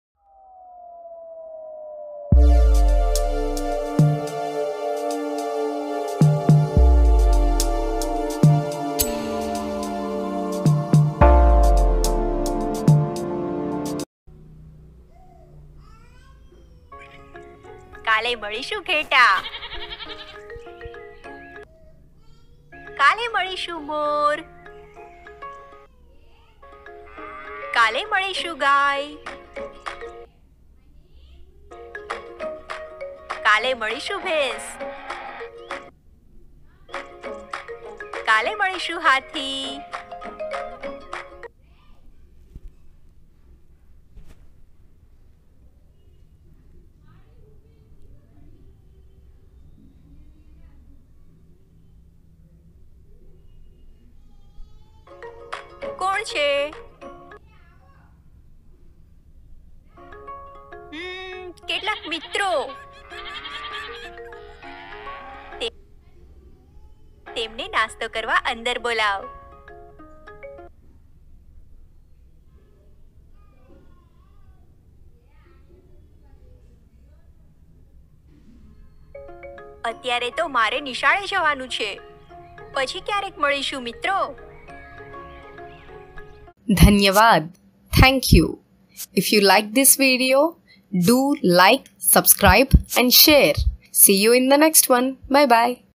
GUJARATI STORIES - કાલે મળીશું SEE YOU TOMORROW | SHORT STORIES TO LEARN AND IMPROVE
Learn and enjoy the short story. You can know that
તમે ક્યારેય કોઈને ‘કાલે મળીશું’ કહ્યું છે અને મળ્યા છો ખરા? આ છોકરાના મિત્રો તો ખરેખર જ મળશે!

In English :
Have you ever said and met someone 'we will meet tomorrow'? This boy's friends will actually be found.